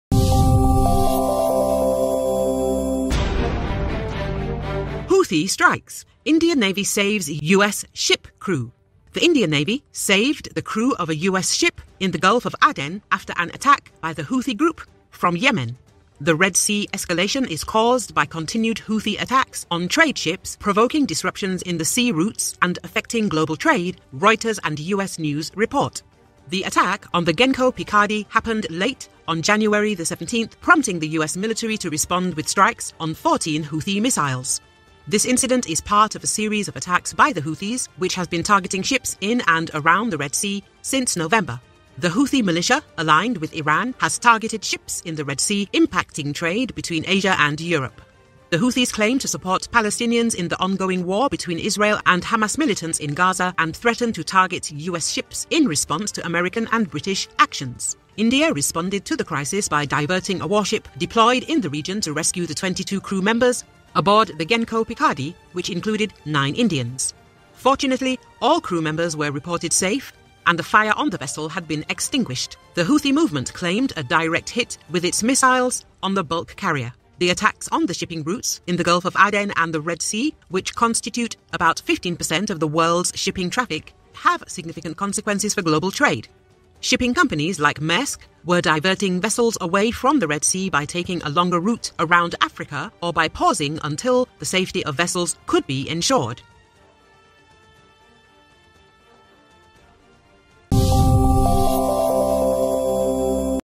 Houthi strikes: Indian Navy saves US ship crew
The Indian Navy saved the crew of a U.S. ship in the Gulf of Aden after an attack by the Houthi group from Yemen. The Red Sea escalation is caused by continued Houthi attacks on trade ships, provoking disruptions in sea routes and affecting global trade, Reuters and US News report.
The attack on the Genco Picardy happened late on January 17, prompting the U.S. military to respond with strikes on 14 Houthi missiles. This incident is part of a series of attacks by the Houthis, which has been targeting ships in and around the Red Sea since November.
The Houthi militia, aligned with Iran, has targeted ships in the Red Sea, impacting trade between Asia and Europe. The Houthis claim to support Palestinians in the ongoing war between Israel and Hamas militants in Gaza and threaten to target U.S. ships in response to American and British actions.
India responded to the crisis by diverting a warship deployed in the region to rescue the 22 crew members aboard the Genco Picardy, which included nine Indians. Fortunately, all crew members were reported safe, and a fire on the vessel had been extinguished. The Houthi movement claimed a "direct hit" with its missiles on the bulk carrier.
The attacks on shipping routes in the Gulf of Aden and the Red Sea, which constitute about 15% of the world's shipping traffic, have significant consequences for global trade. Shipping companies like Maersk were diverting vessels away from the Red Sea by taking a longer route around Africa or by pausing until the safety of vessels could be ensured.